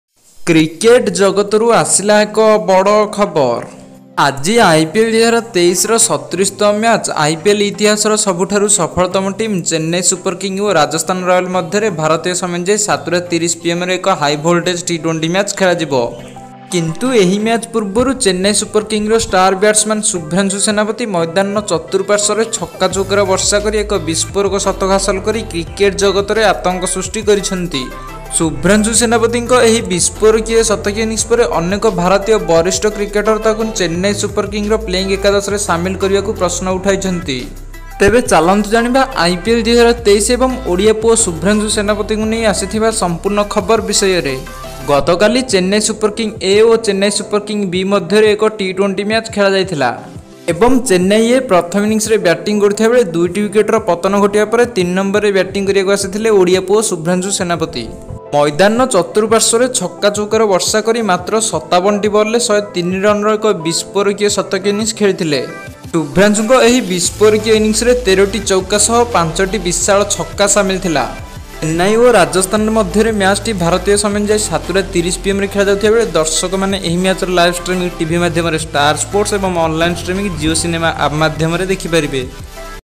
Odia Cricketer Subranshu Senapati Likely To Debut in CSK Next Match | Cricket News Odia
Odia Cricketer Subranshu Senapati Likely To Debut in CSK Next Match | Cricket News Odia @cricketnewsodiaa